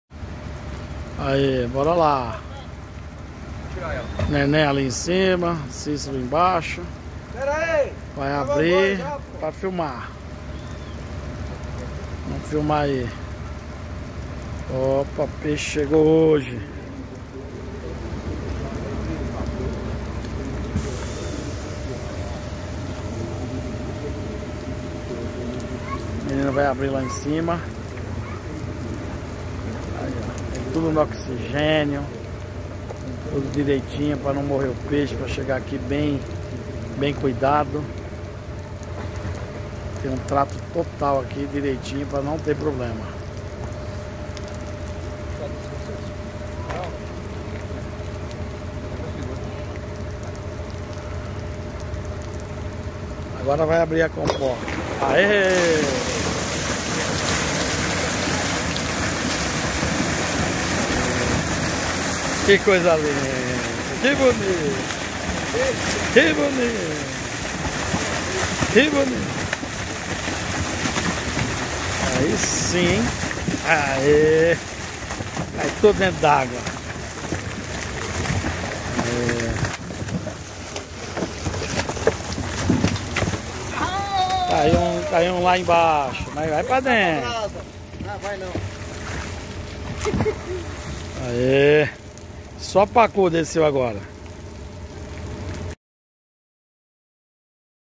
PESQUEIRO RECEBE PEIXES PRA SEMANA SANTA
O Pesqueiro do Clube de Campo garantirá peixe fresco e de qualidade pra Semana Santa. O local receberá 5.200 quilos de várias espécies. No sábado (24) chegou a primeira remessa. Explica Fuminho, administrador do Clube: “Recebemos 2.600 quilos de tilápia e pacu. Até a Semana Santa serão mais 2.600 quilos, principalmente de tilápia”. Narração do Chorão...